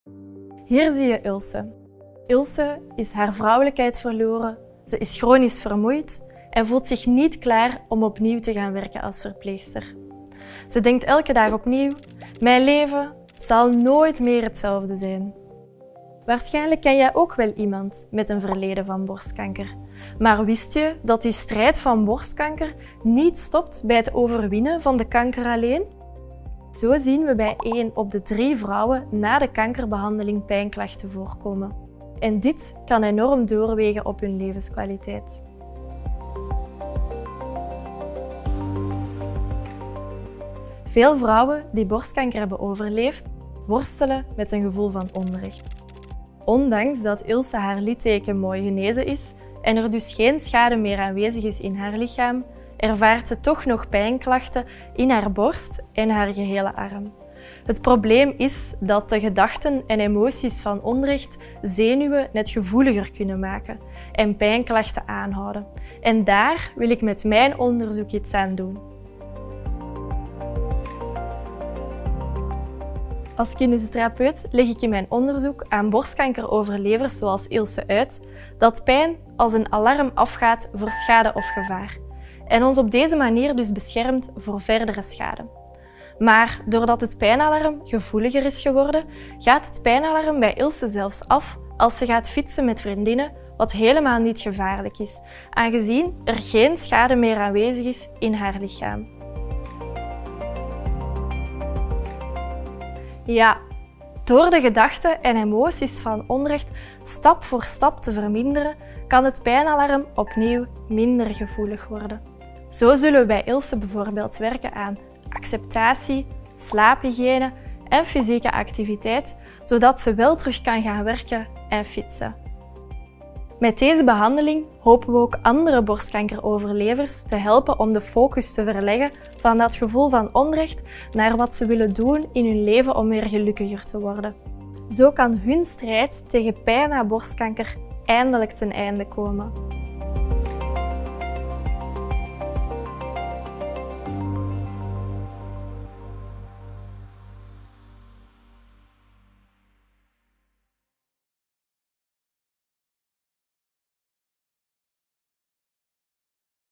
De strijd tegen pijn na borstkanker
De strijd tegen borstkanker stopt niet bij het overwinnen van de kanker. Bij 1 op de 3 vrouwen is hun 'pijn-alarm' ontregeld, waardoor ze zelfs bij een onschuldige activiteit zoals fietsen pijnklachten blijven ervaren. Kinesitherapeute  Eva Roose (VUB) wil deze borstkankeroverlevers helpen hun pijn-alarm te resetten.